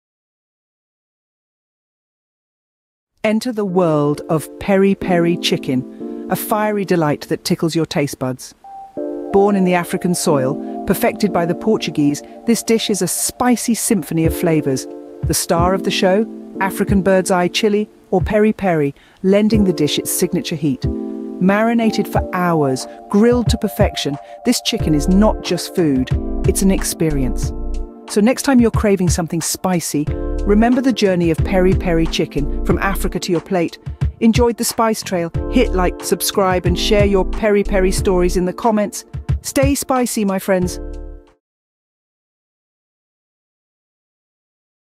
Peri Peri Chicken A Spicy World
Whet your appetite with our delectable video guide on creating the perfect Peri-Peri Chicken – a zesty fusion of spices that's sure to set your senses alight! 🌶️🍗 Learn from experts how Portugal's beloved dish can easily be recreated in your kitchen. Hungry for more? Hit ‘play’ and let’s get cooking! 👍💬🔔